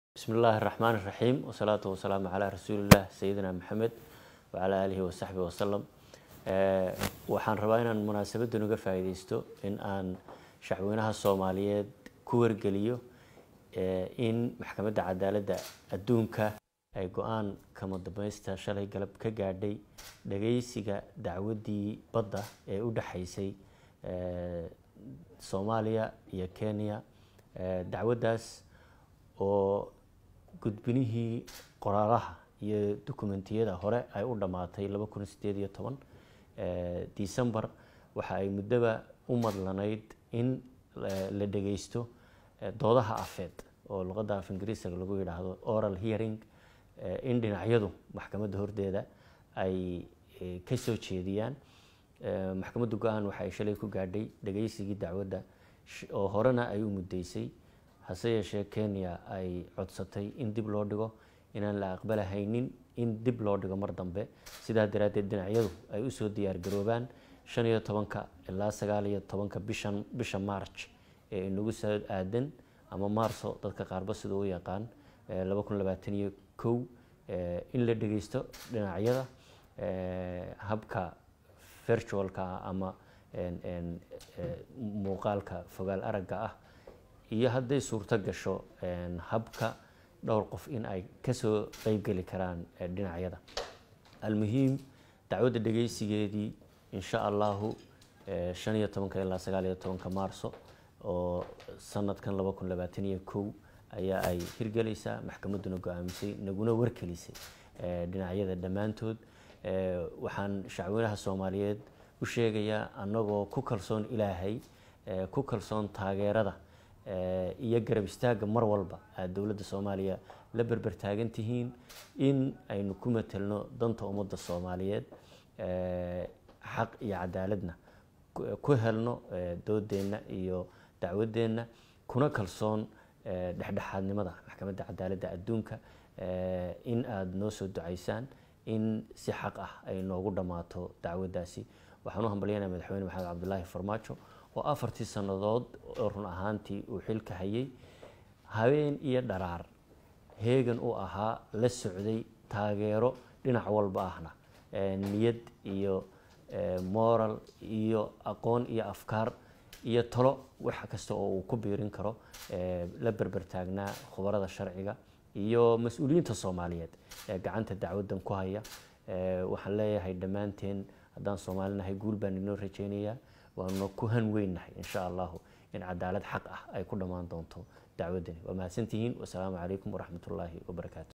Maxkamadda ICJ oo diiday Codsigii Kenya ee dib u dhigista Dacwada Badda.
Ra’iisul Wasaare ku-xigeenka Xukuumadda Federaalka Soomaaliya ee xilka sii-haya Mahdi Maxamed Guuleed Khadar ayaa ka hadlay go’aan Maxkamadda Cadaalada aduunka ee ICJ ka gaartay Dacwada badda ee Soomaaliya iyo Kenya.
Mahdi Guuleed oo warbaahinta la hadlay ayaa waxaa uu sheegay in Maxkamadda ICJ shalay go’aan kama dambeys ah ku gaartay in waqtigii horey u balamisay in la dhageysto kiiska badda la dhageysan doono, isla markaana aanu jirin doonin dib u dhigis kale.